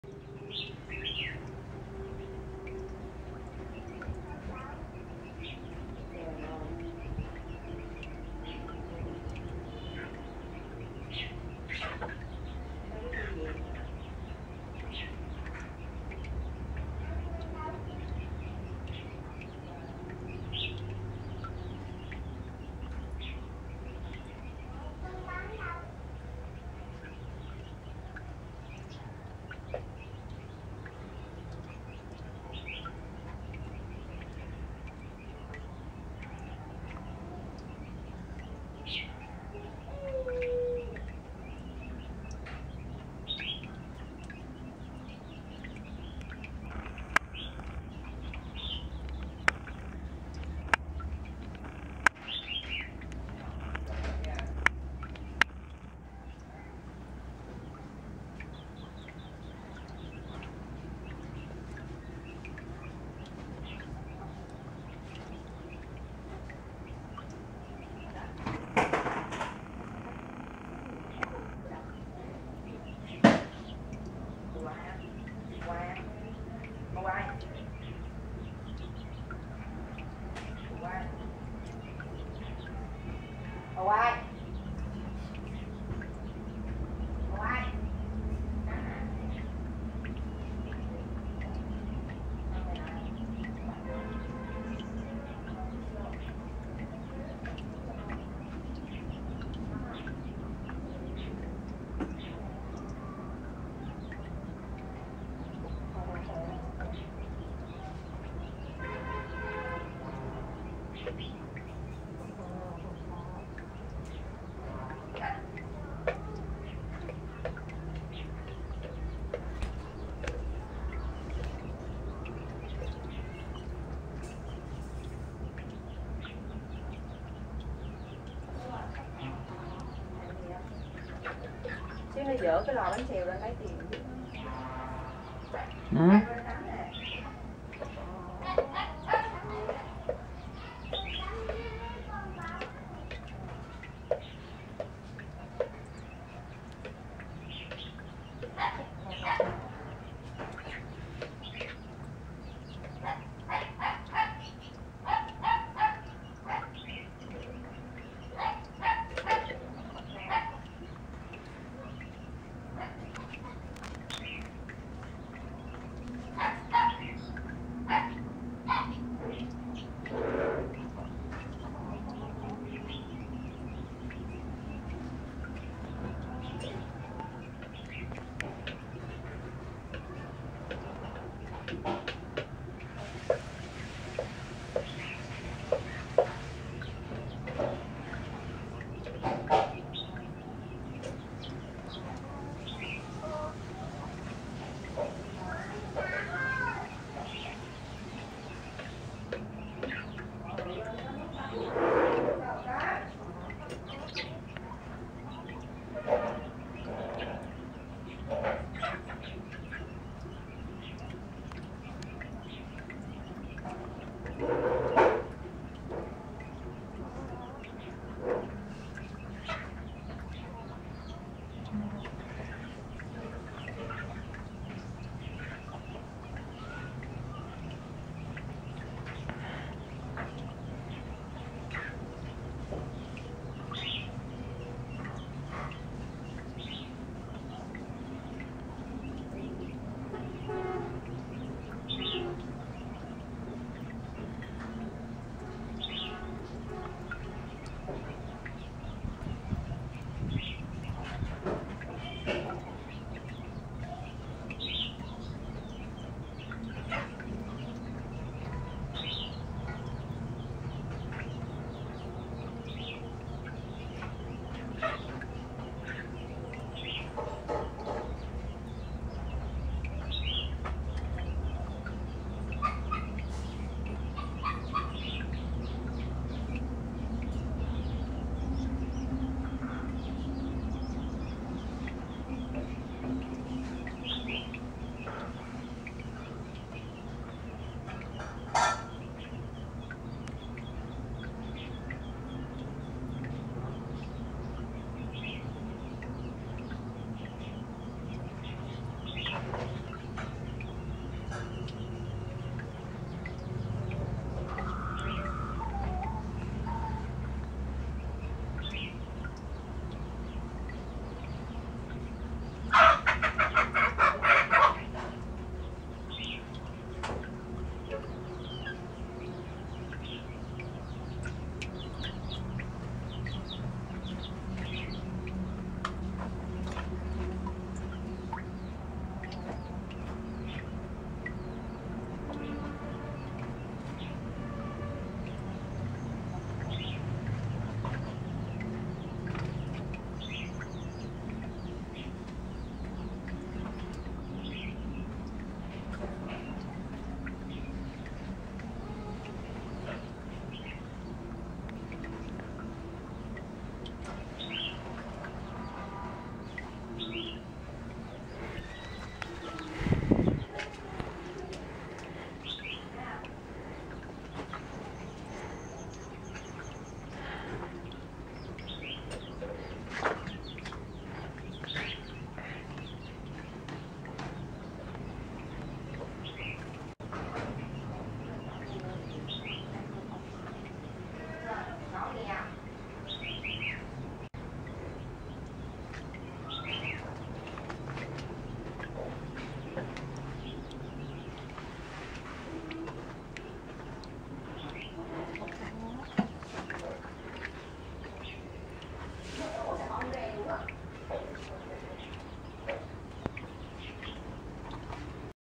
Hồ cá không cần dàn lọc vẫn trong veo ( P1 )
Hồ cá nhà mình không cần dàn lọc và cá vẫn khỏe mạnh và nước lúc nào cũng trong veo hết!
Trong hồ gồm cá bảy màu, cá ba đuôi, cá koi và cá koi bướm, cá bạch kim